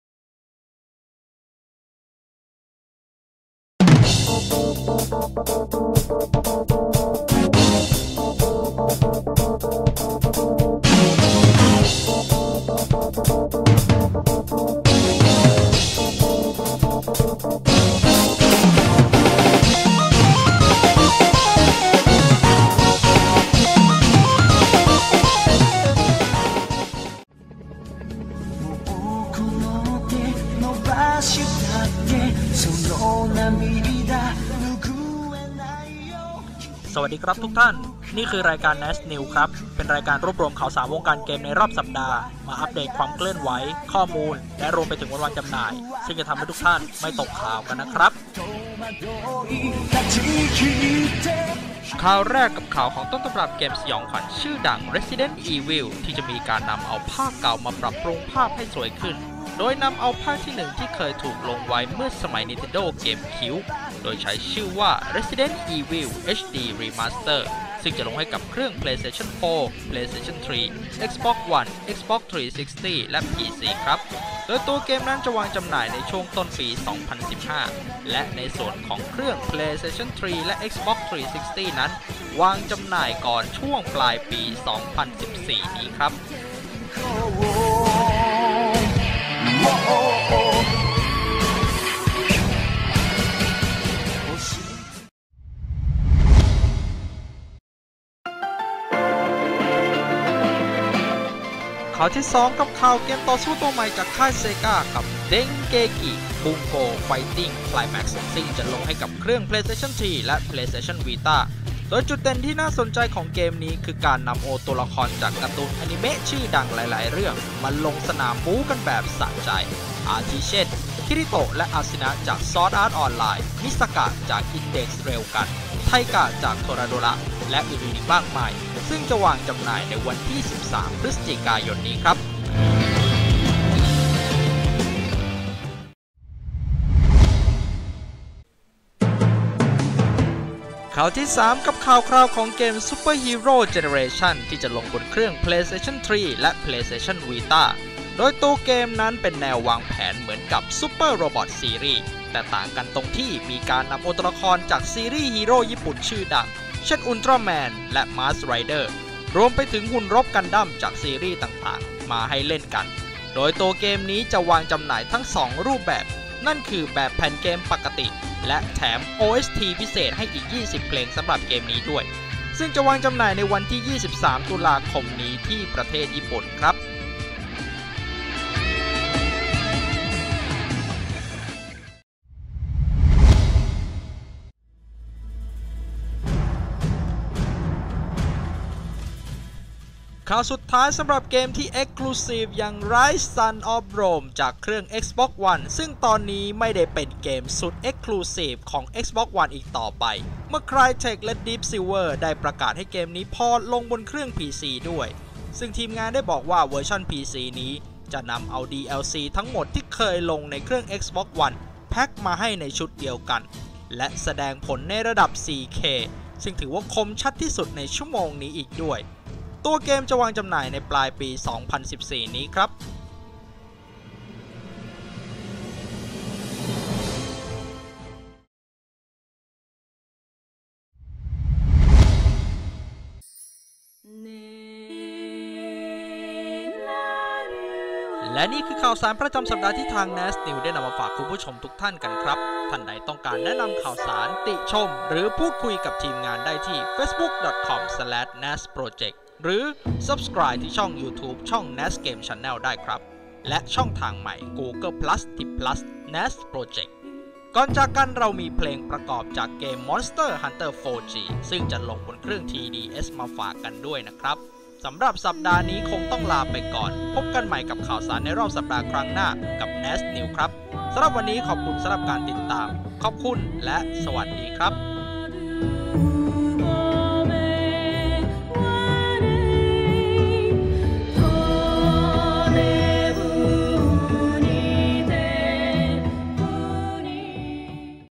Nadz News 4 - 10 Aug 2014
Nadz News รายการข่าวที่จะมารายงานข่าวสารแวดวงการเกมส์ที่น่าสนใจประจำสัปดาห์
สัปดาห์นี้มีข่าวที่น่าสนใจ 4ข่าวดังนี้
[0.27] Resident Evil HD Remaster ลงทุกเครื่องในช่วงปี2015
[1.37] รวมดาวอนิเมะ Denki Bunko Fighting Climax
[2.18] ข่าวคืบหน้าของ Super Hero generations
[3.05] Ryse Son Of Rome ไม่ Exclusive แล้ว
ทั้งหมดนี้อยู่ในNadz Newsประจำสัปดาห์ของวันที่ 4- 10 สิงหาคม 2557